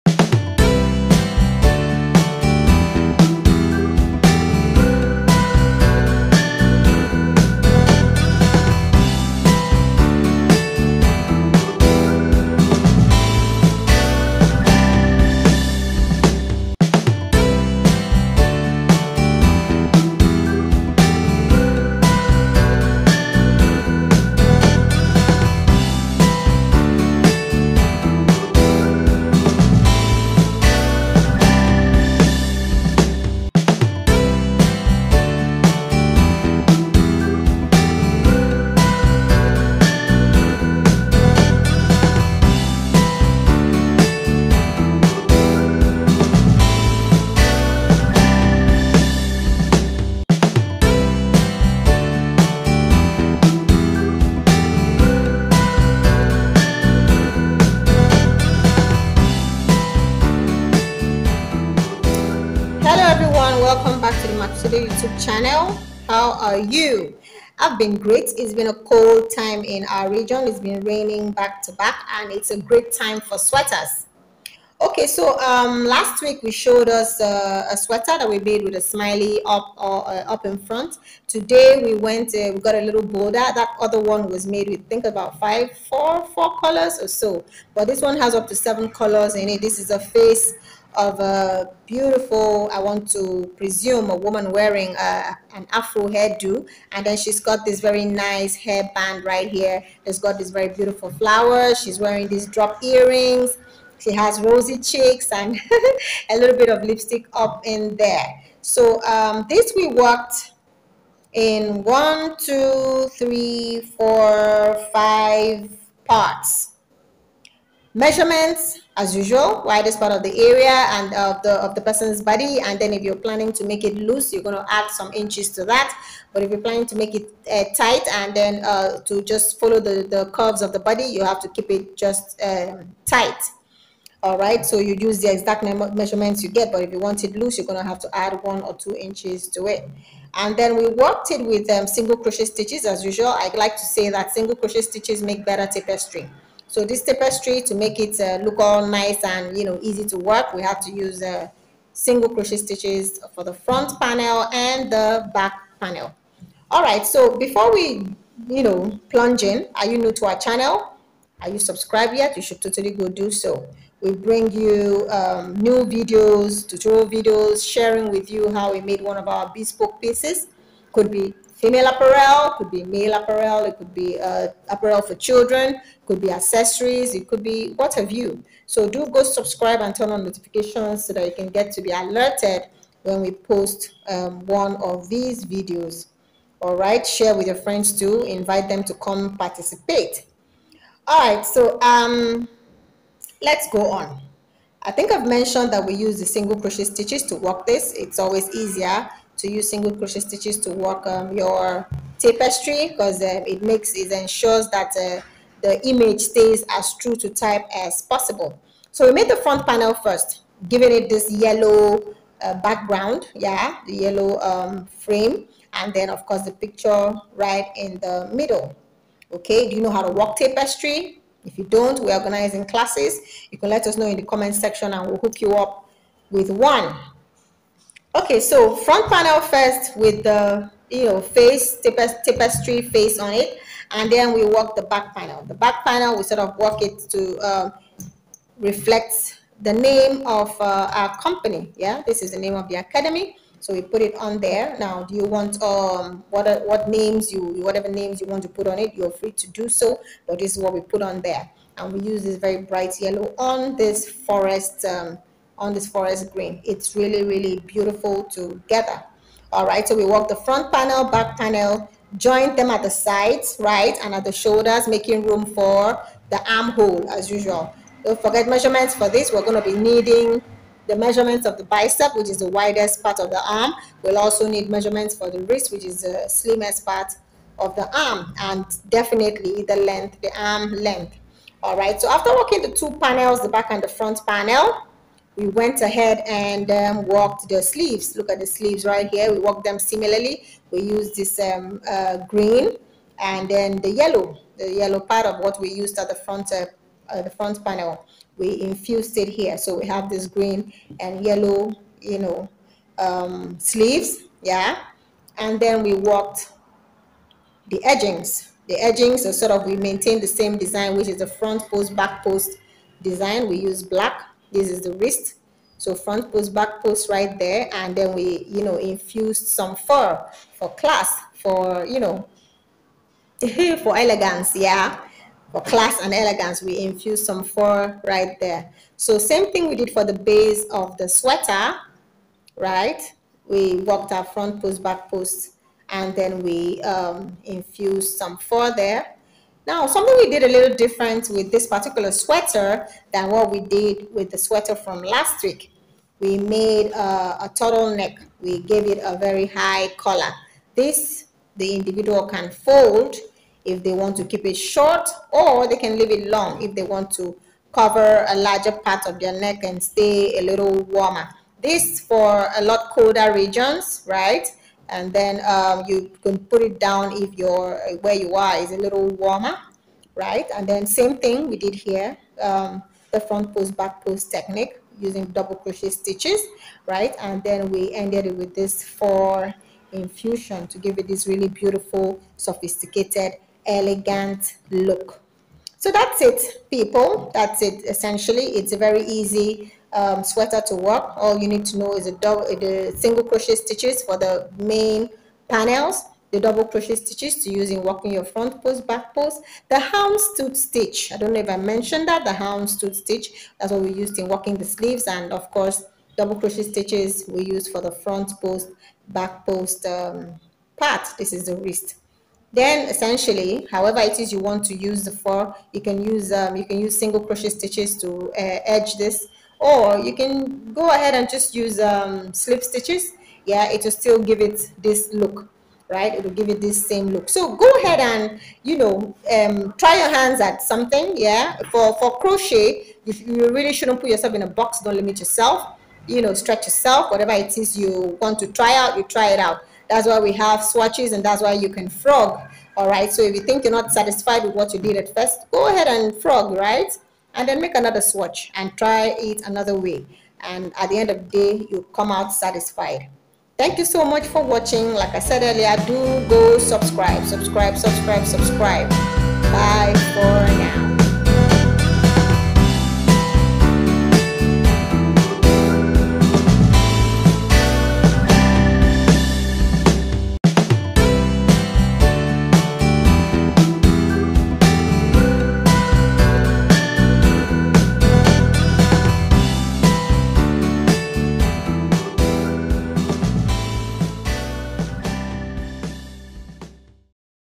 Afro Princess, Turtle Neck, Tapestry Sweater With Houndstooth Stitches For Women ~ June 2023
Our 'Afro Princess' Sweater for Women was worked in 5 parts.

Stitches used:
🧶 Sc stitch
🧶 Houndstooth stitch
🧶 Dc fpbp stitch

🧶The Body: Worked in two separate panels with single crochet stitches and in different colours. The front panel has the smiley (written pattern available)

We joined both panels making room for the head and arms.

🧶The Sleeves: We worked long sleeves using the Houndstooth Stitch

🧶Sleeves and Base edgings: We worked these with dc fpbp stitches. We did same for the very high collar. We spiced the edges with fur.

Body measurements required to work this sweater are:

✔️Length of sweater
✔️Widest part of the body
✔️Arm hole depth
✔️Long Sleeve length
✔️Bicep
✔️Wrist

Videos on how to do basic tapestry.

Very easy to follow for  Intermediate and Advanced Crochet skill levels.

Note that this same technique can be replicated for any size just by adjusting the measurements.

Do Watch, Listen, Read and Drop questions below if any.

You may ask for guidance on specifics parts of the video and also suggest tutorials you would love to see on here.

to get inspired by our crochet gallery.

to get acquainted with our other businesses.